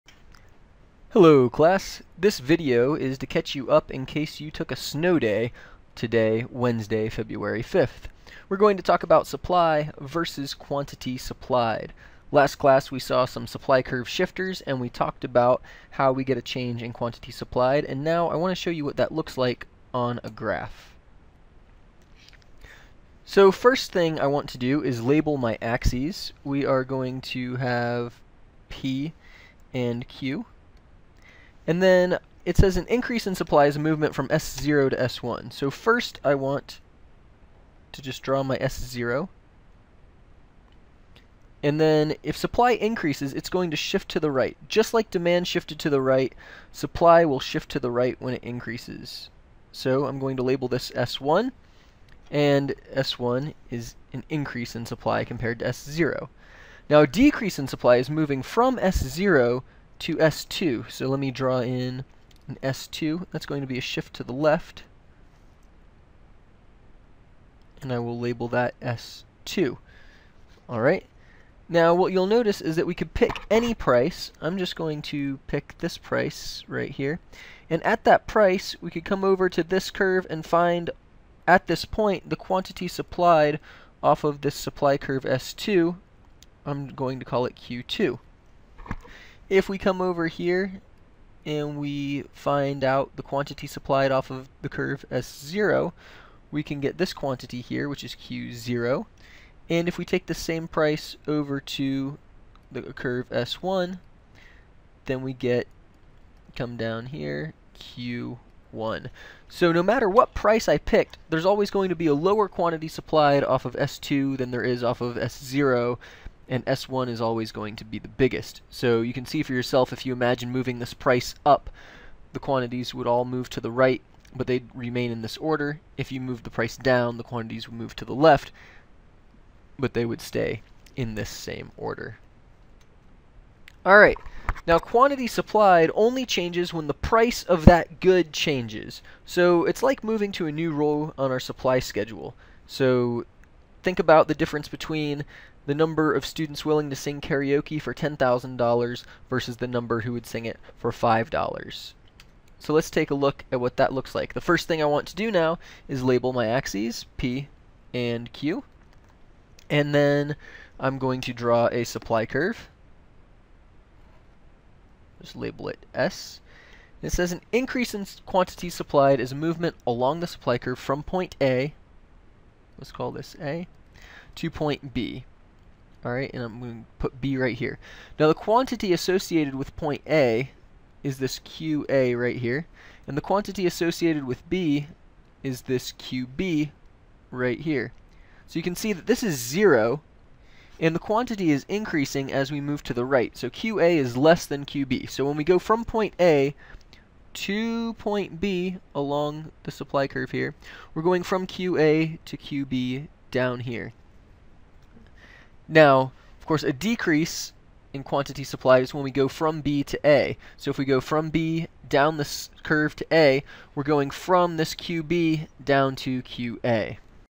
Supply vs Quantity Supplied
In case you took a snow day, you missed this if you were in the 9:05am class for ECON102.  If you're in the later class, this might be good review.